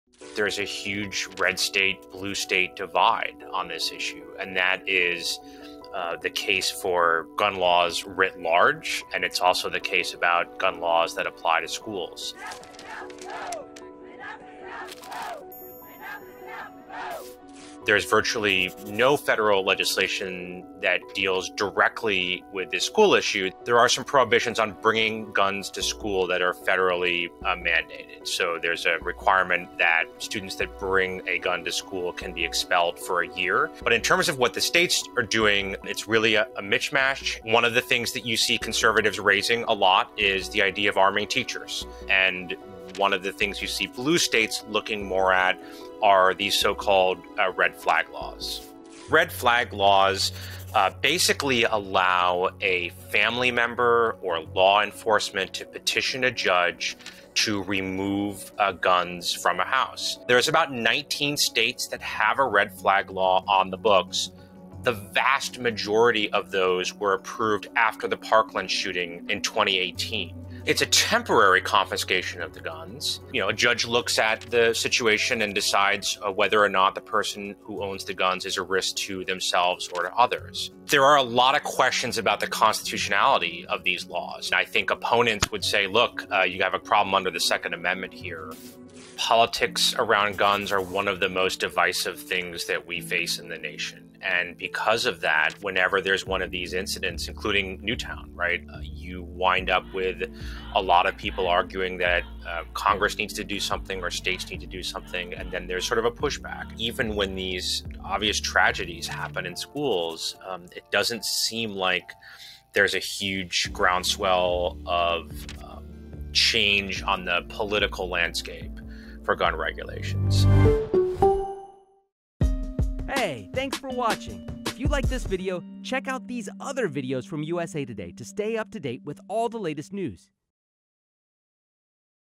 School shootings: What gun legislation looks like across the US | JUST THE FAQS
Here's why we are still seeing school shootings across the US.

From arming teachers to red flag laws, here is some of the legislation being discussed in the wake of school shootings.